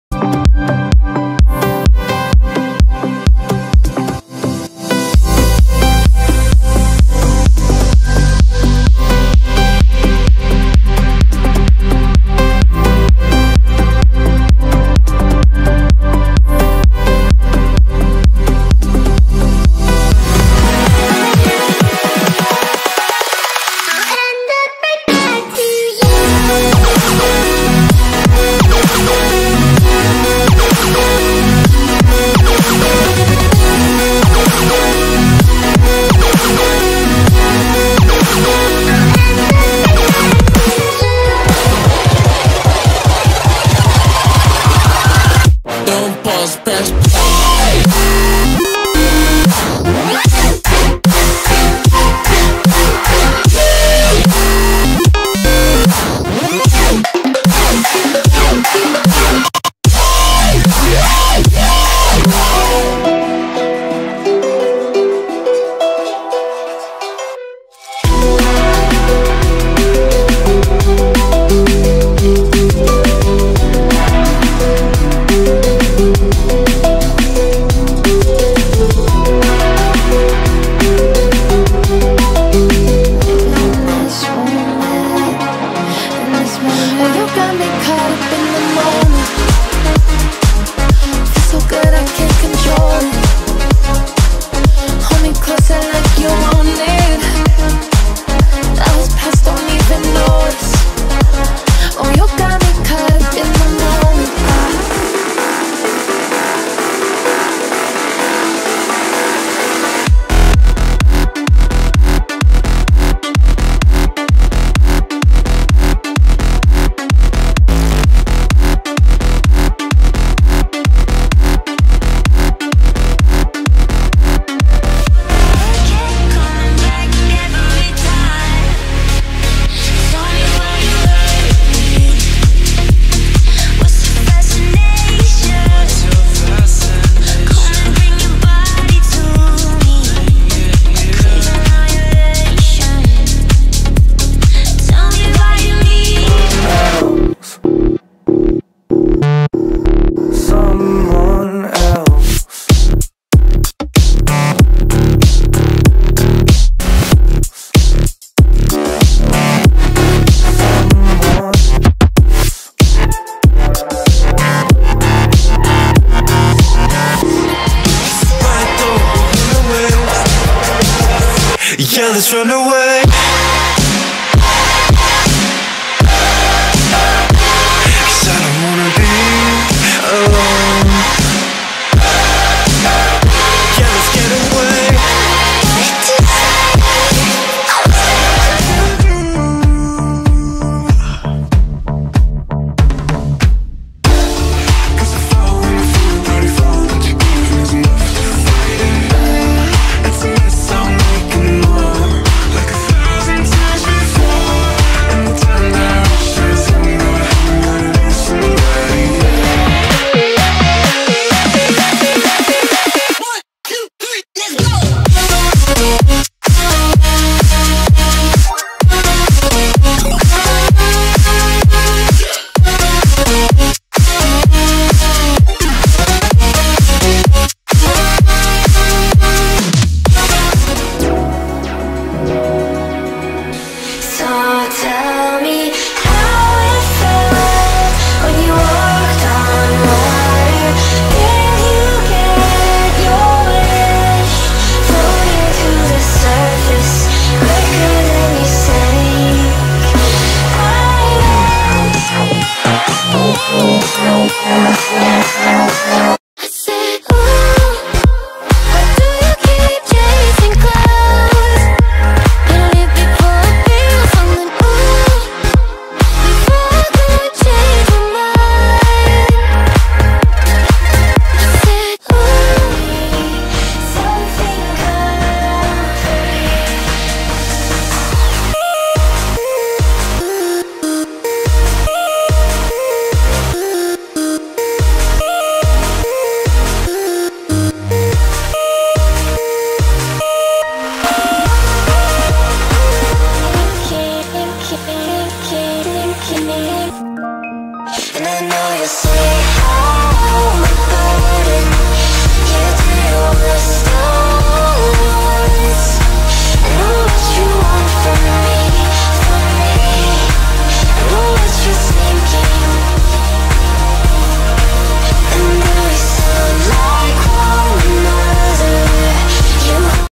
The Mezzanine Ranks Eswyn's Top 15 Songs of 2020
**Warning: This video contains flashing lights which may not be suitable for people who suffer from photosensitive epilepsy.**

Follow our members on social media!